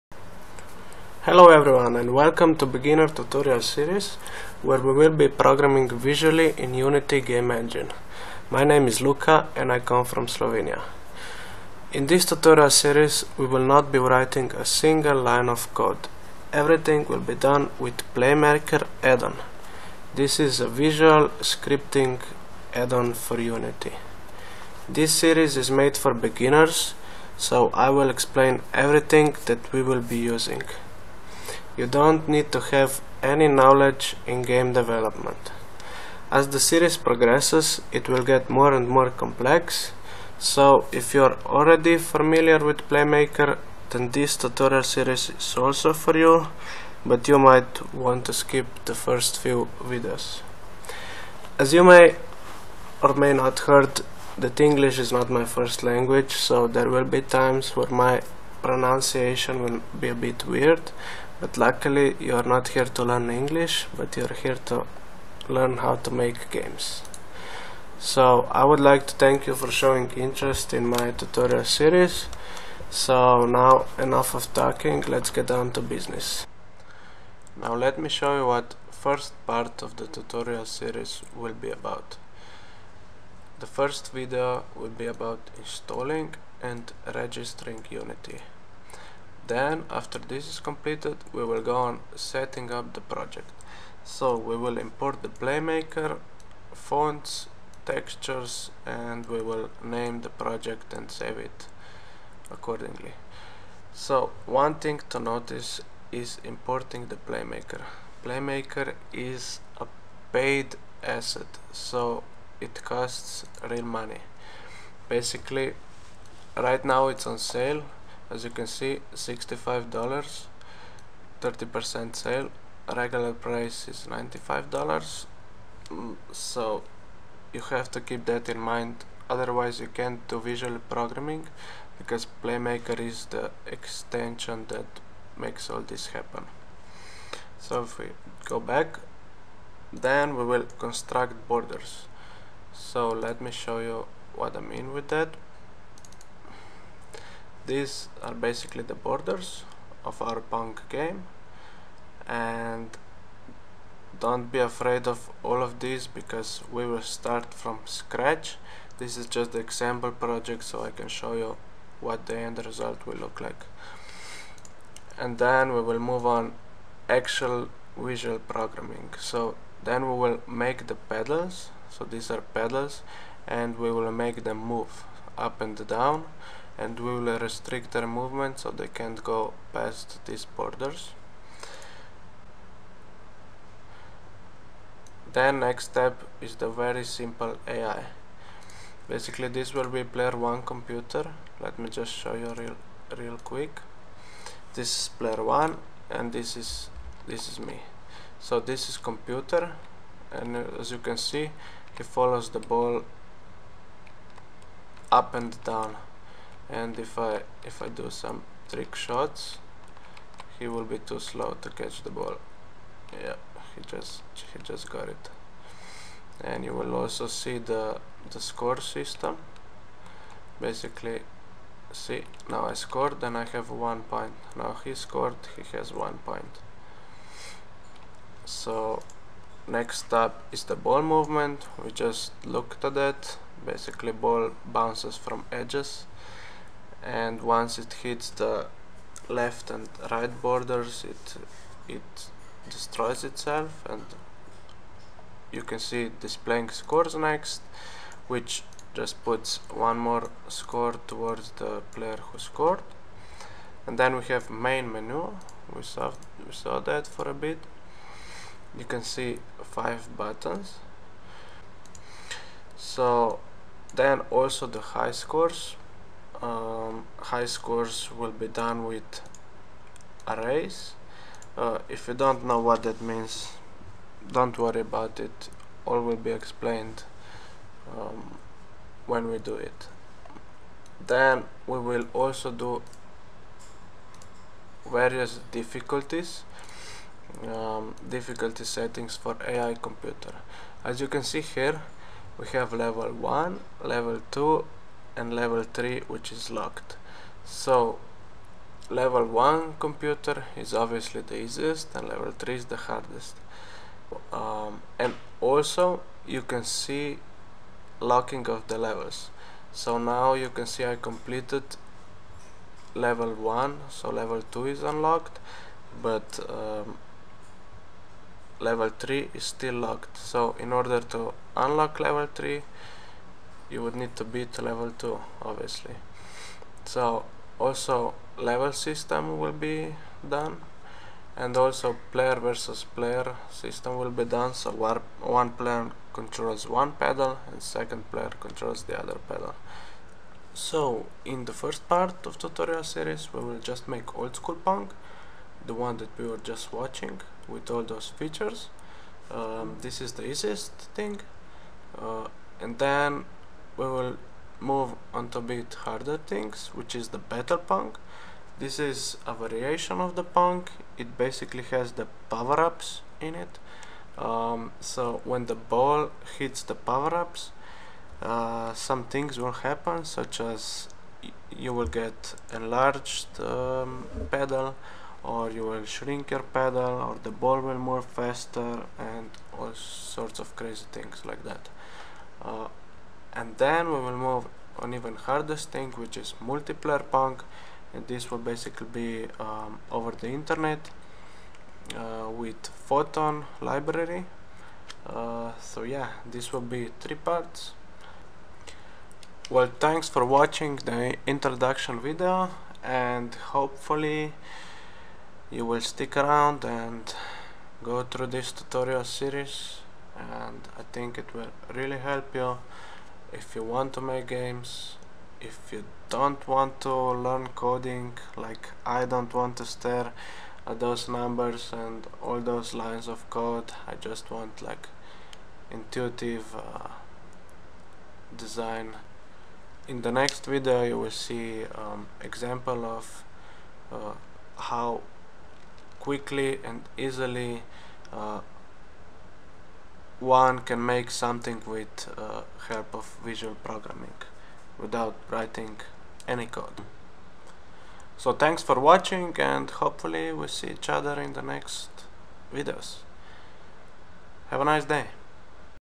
Beginner Visual Programming Tutorial Series - Unity 3D & Playmaker #1
Introduction to my tutorial series for beginners, where we will use visual programming to first make a simple pong game.

Then we will make battle pong, which contains powerups, and lastly we will make multiplayer pong with help of photon multiplayer library.